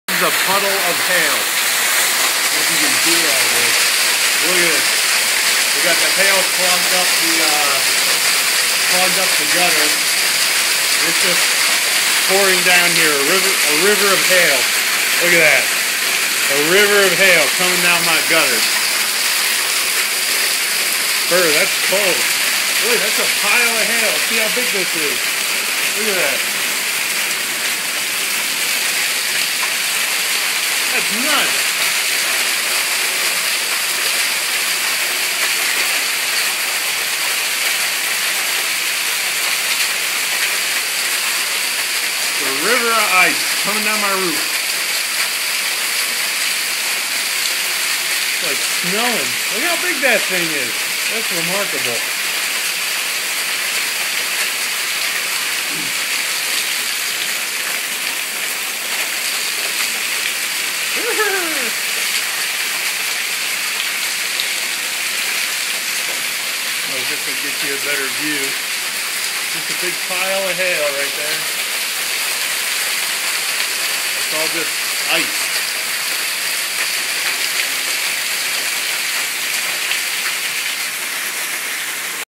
River of hail
Had some hail Jan 20. The hail clogged my gutter so it was just pouring down off the roof into a huge pile on my porch.